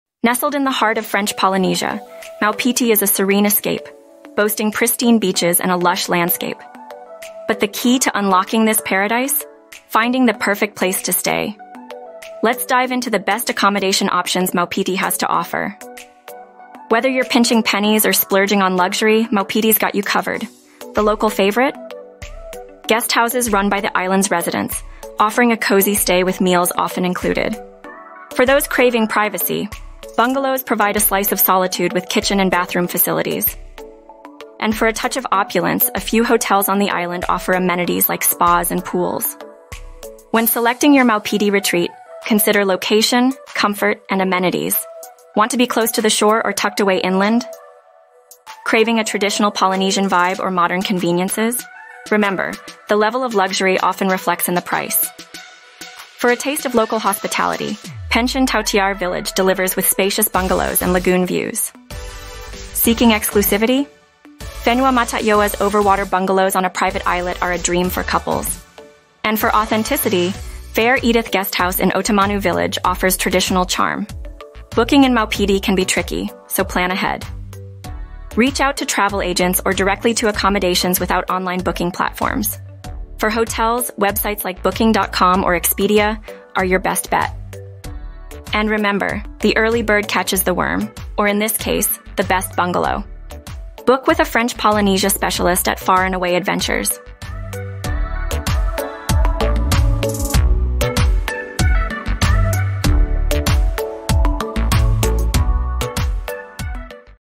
Find Your Perfect Stay in Maupiti | Top 5 Accommodation Picks in French Polynesia's Hidden Gem
Embark on a journey to the serene and secluded island of Maupiti in French Polynesia, a paradise for those seeking peace and tranquility. With its stunning beaches, crystal-clear waters, and lush landscapes, Maupiti is an ideal destination for travelers dreaming of a quiet escape. Finding the right accommodation is key to making your trip unforgettable, and that's where we come in! In this video, we dive into five essential subtopics about Maupiti accommodations, guiding you to discover the perfect place to stay. Whether you're looking for luxury resorts, cozy bungalows, or budget-friendly options, we've got you covered. Get ready to unlock the secrets of Maupiti's best accommodation choices and plan the vacation of your dreams!